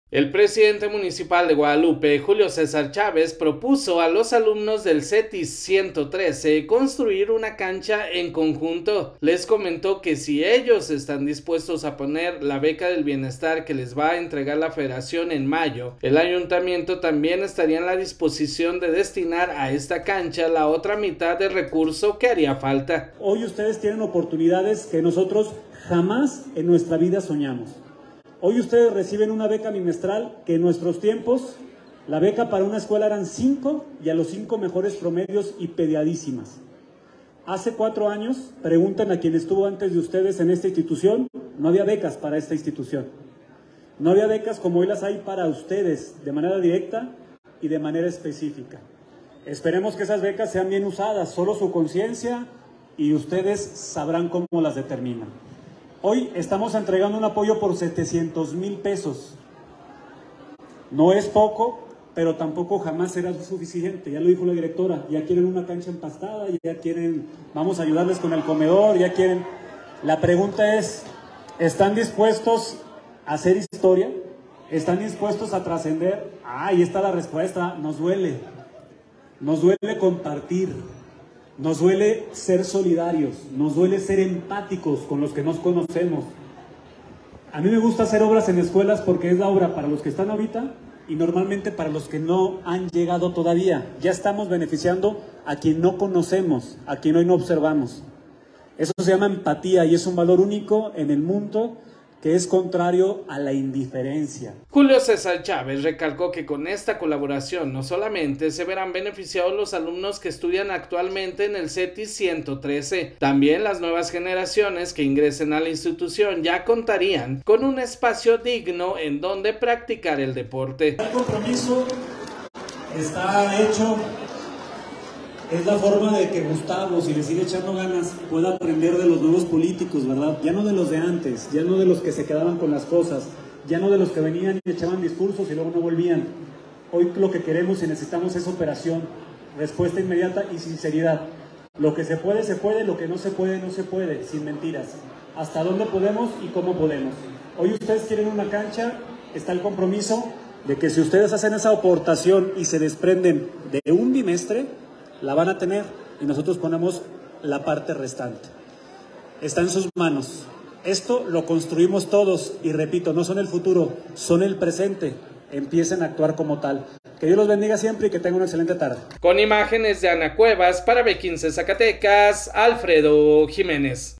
Propone alcalde de Guadalupe a alumnos del CETIS 113 hacer una cancha de futbol de manera conjunta
Zacatecas, Zacatecas. – El presidente municipal de Guadalupe, Julio César Chávez propuso a los alumnos del CETIS 113 construir una cancha en conjunto, les comentó que, si ellos están dispuestos a poner la beca del Bienestar que les va a entregar la Federación en mayo, el Ayuntamiento también estaría en la disposición de destinar a esta cancha la otra mitad de recurso que haría falta.

Julio César Chávez recalcó que con esta colaboración no solamente se verán beneficiados los alumnos que estudian actualmente en el CETIS 113, también las nuevas generaciones que ingresen a la institución ya contarían con un espacio digno en donde practicar el deporte.